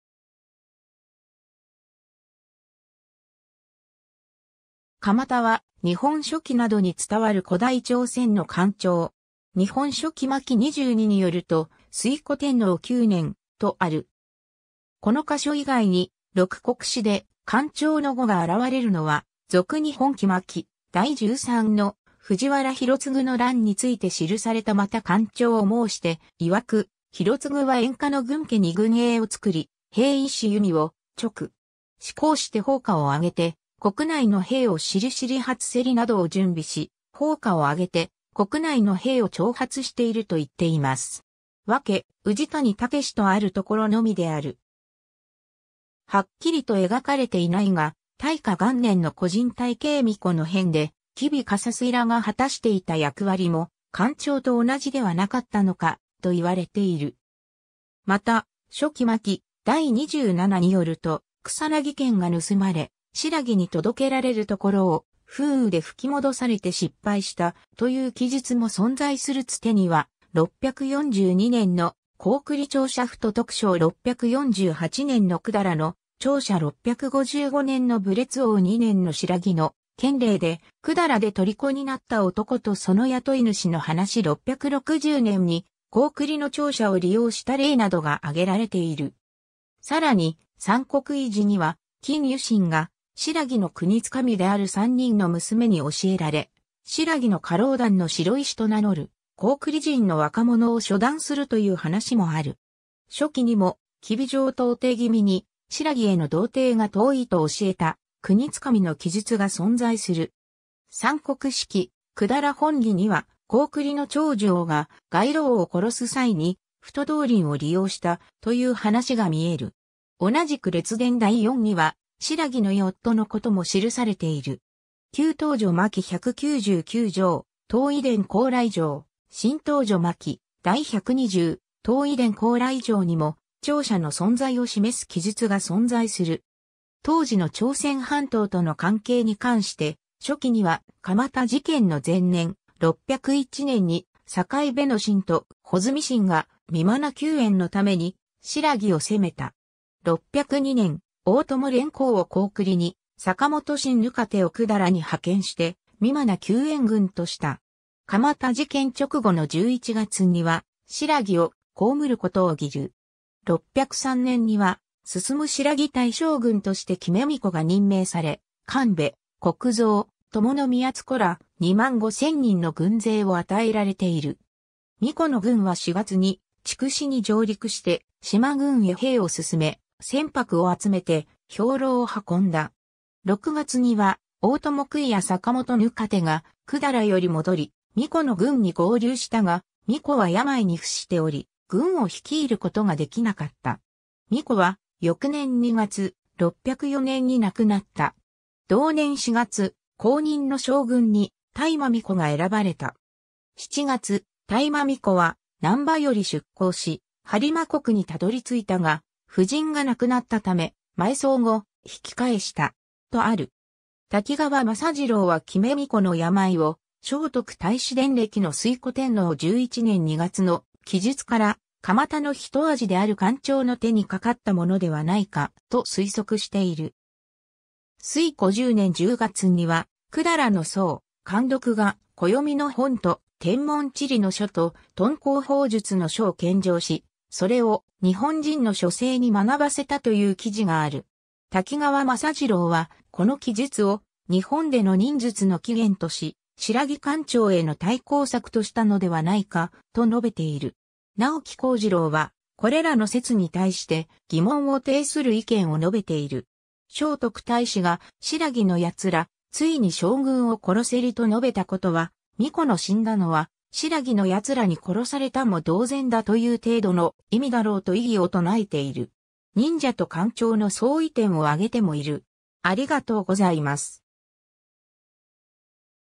迦摩多
迦摩多（かまた、生没年不詳）は、『日本書紀』などに伝わる古代朝鮮の間諜。
 『日本書紀』巻二十二によると、推古天皇9年（454年）、 　　　　　　　　　　　　　　　　　　　　　　　　　　　 とある。
この箇所以外に、「六国史」で「間諜」の語が現れるのは、『続日本紀』巻第十三の、藤原広嗣の乱について記された また間諜（うかみ）申して云（い）はく、「広嗣（ひろつぐ）は遠珂（をか）の郡家（ぐんけ）に軍営（ぐんゐやう）を造（つく）り、兵弩（ひゃうど）を儲（まう）く。而（しか）して烽火（とぶひ）を挙（あ）げて、国内（くぬち）の兵（いくさ）を徴（はた）り発（おこ）せり」 （また、間諜の報告によると、広嗣は遠賀郡の群家に軍営を造って、弩（いしゆみ）などを準備し、烽火（のろし）をあげて国内の兵を徴発しているといっています。
）訳：宇治谷孟 とあるところのみである。
 はっきりと描かれていないが、大化元年（645年）の古人大兄皇子の変で、吉備笠垂らが果たしていた役割も、間諜と同じではなかったのか、と言われている。
 また、『書紀』巻第二十七によると、草薙剣が盗まれ、新羅に届けられるところを、風雨で吹き戻されて失敗した、という記述も存在する（草薙剣盗難事件。 『万葉集』巻第二、109番の歌で、大津皇子は津守通の占いで出た結果にかかわらず、石川郎女と関係をもったという趣旨の歌を詠んでいる。この津守通が果たした役割が間諜であった可能性がある、と吉永登は述べている。 日本における記録はここまでであるが、大陸や半島の史書には間諜の記録が若干あり、『三国史記』の金庾信（きんゆしん）伝には、 642年（善徳王12年）の「高句麗諜者浮屠徳昌（ふととくしょう）」 648年（真徳王の太和2年）の百済の諜者 655年（永徽6年）の武烈王2年の新羅の県令で百済で虜になった男とその雇い主の話 660年（龍朔元年）に高句麗の諜者を利用した例 などがあげられている。
 さらに『三国遺事』には、金庾信が新羅の国つ神である3人の娘に教えられ、新羅の花郎団の白石と名乗る高句麗人の若者を処断するという話もある。
『書紀』にも、吉備上道弟君に、新羅への道程が遠いと教えた「国神」の記述が存在する。
 『三国史記』「百済本紀」には、高句麗の長寿王が蓋鹵王（こうろおう）を殺す際に、浮屠道琳を利用した、という話が見える。
同じく列伝第四には新羅の居柒夫（きょしつふ）のことも記されている。
 『旧唐書』巻一百九十九上・東夷伝高麗条、『新唐書』巻第百二十、東夷伝高麗条にも、諜者の存在を示す記述が存在する。
 当時の朝鮮半島との関係に関して『書紀』には、 迦摩多事件の前年、601年に境部臣と穂積臣が任那救援のために新羅を攻めた。
 602年（推古天皇10年3月）、大伴連噛を高句麗に、坂本臣糠手を百済に派遣して、任那救援軍とした。
 迦摩多事件直後の11月には「新羅（しらき）を攻（せ）むることを議（はか）る」。
 603年（推古天皇10年2月）には征新羅大将軍として来目皇子が任命され、神部（かんとものを）・国造・伴造ら、2万5千人の軍勢を与えられている。
 皇子の軍は4月に筑紫に上陸して、嶋郡（しまのこおり、現在の福岡県糸島市北半分）へ兵をすすめ、「船舶」（つむ）をあつめて、兵粮を運んだ。
 6月には大伴咋や坂本糠手が百済より戻り、皇子の軍に合流したが、皇子は病に臥しており、軍を率いることができなかった。
 皇子は翌年2月、604年になくなった。
 同年4月、後任の将軍に当摩皇子が選ばれた。
 7月、当摩皇子は難波より出航し、播磨国に辿り着いたが、夫人がなくなったため、埋葬後、引き返した。
 とある。
 滝川政次郎は来目皇子の病を、『聖徳太子伝暦』の推古天皇11年2月の記述から、迦摩多の一味である間諜の手にかかったものではないか、と推測している。
 推古10年10月には、百済の僧、観勒（かんろく)が暦の本と天文地理の書と、遁甲方術の書を献上し、それを日本人の書生に学ばせたという記事がある。滝川政次郎はこの記述を日本での忍術の起源とし、新羅間諜への対向策としたのではないか、と述べている。 直木孝次郎は、これらの説に対して、疑問を呈する意見を述べている。聖徳太子が「新羅の奴ら、遂に将軍を殺せり」と述べたことは、皇子の死んだのは新羅の奴らに殺されたも同然だ」という程度の意味だろうと異議を唱えている。忍者と間諜の相違点をあげてもいる。